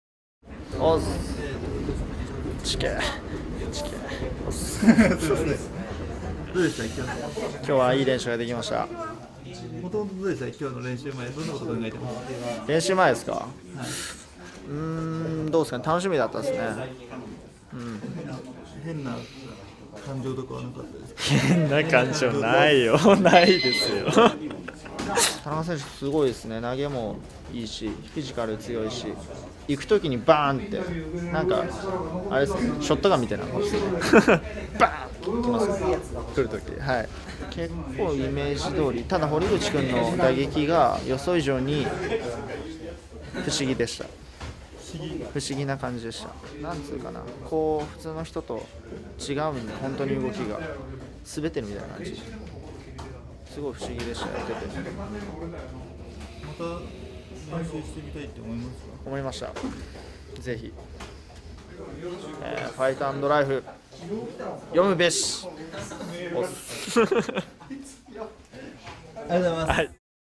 【佐々木憂流迦】堀口恭司&田中路教との合同トレ動画 | Yuta Sasaki trainned with Kyoji Horiguchi & Michinori Tanaka
佐々木憂流迦、堀口恭司、­田中路教、矢地祐介の4人が神奈川県横浜のグランドスラムで合同練習を行った。
Kyoji Horiguchi, Yuta Sasaki & Michinori Tanaka trainned together at GRANDSLAM, yokohama.

この合同練習は格闘技専門誌Fight&Lifeの企画に対し、佐々木と田中の師匠で­ある芹沢健一、勝村周一朗の両氏、そして堀口の先輩である朴光哲の同意の下、13年11月27日（水）に実現した­もの。13年の2月に同誌上で、バンタム級新世代のライバルとして同時に扱った記事のな­かで、3人がそれぞれ強烈なライバル意識を持ち、我こそはナンバーワンという気持ちを­全面に押し出していた。あれから10ケ月が過ぎ、3人の巡る状況も変化し、一言でいえ­ば「強くなる」ために合同練習が実現に至った。